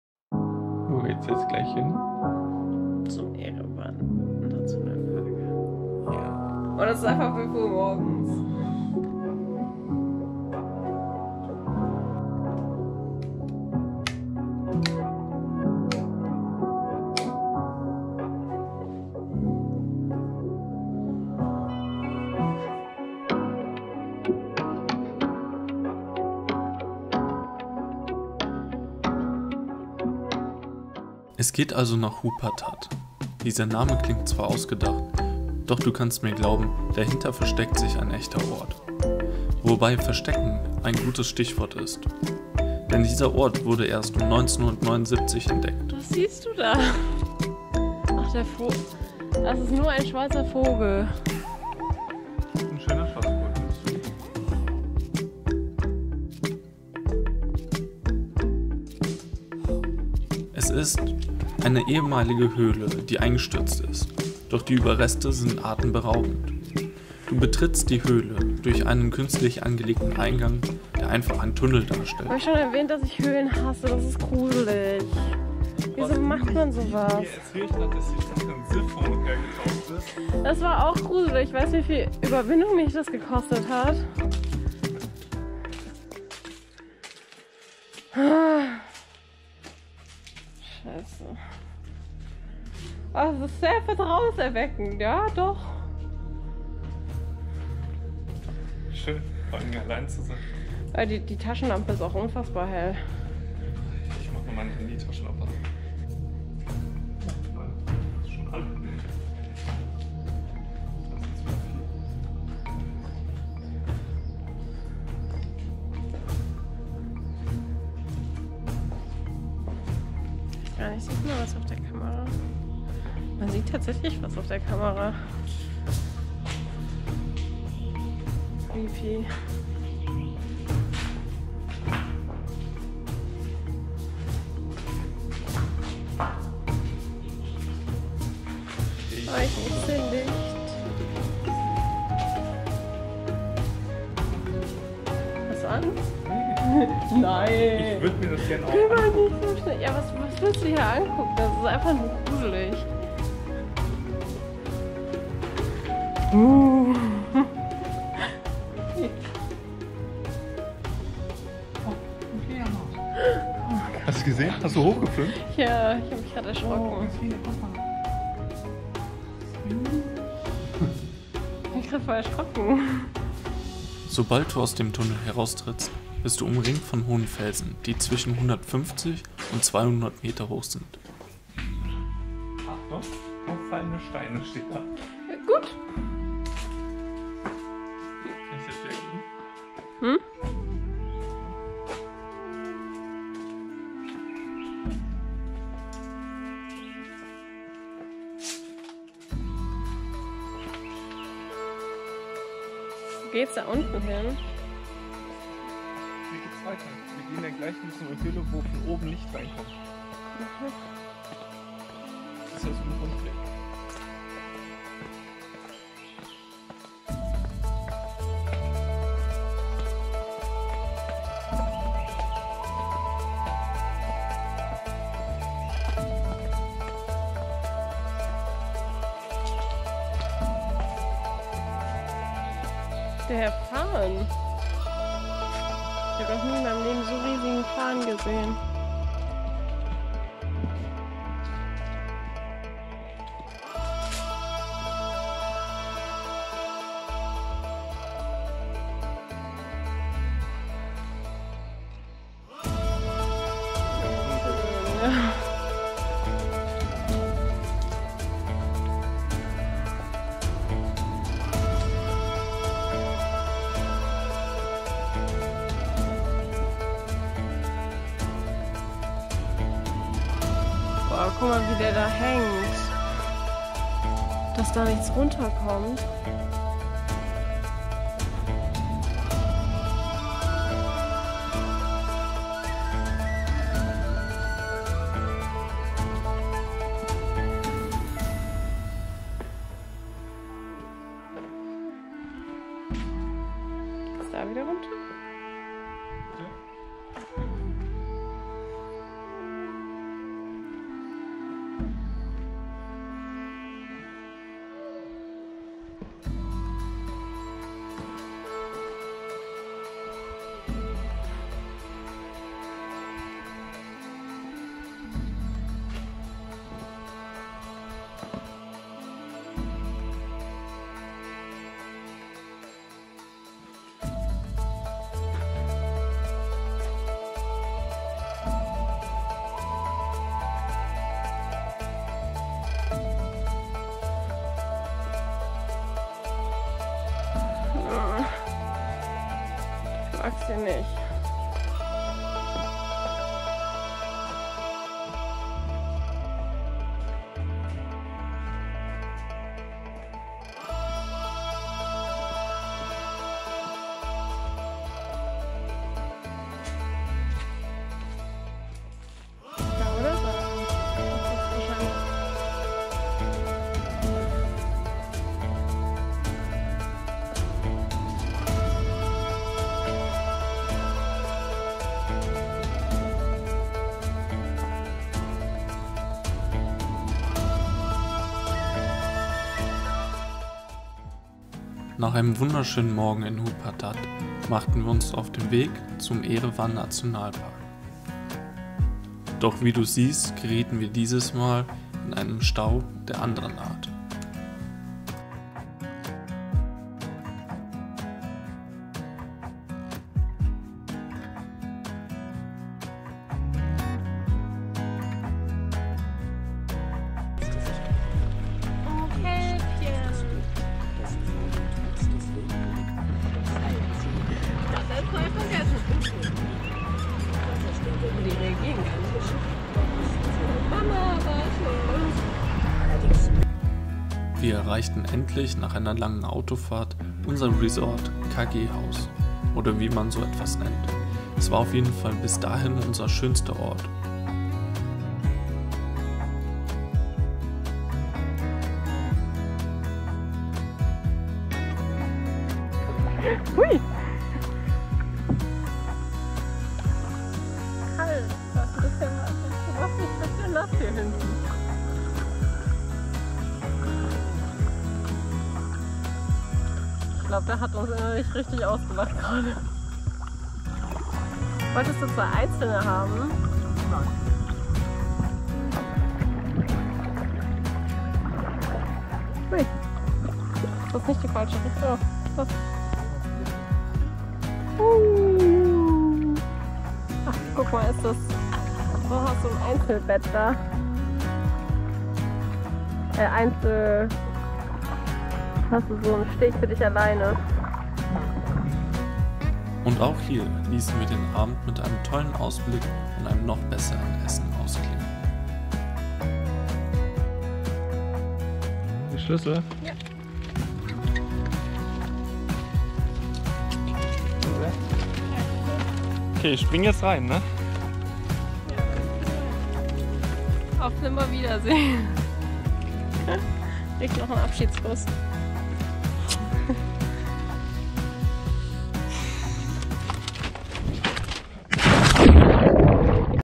Thailand Vlog 19 Hup Pa Tat: Das vergessene Tal
Auf dem Weg zum Erewan National Park machten wir an einem interessanten Ort rast.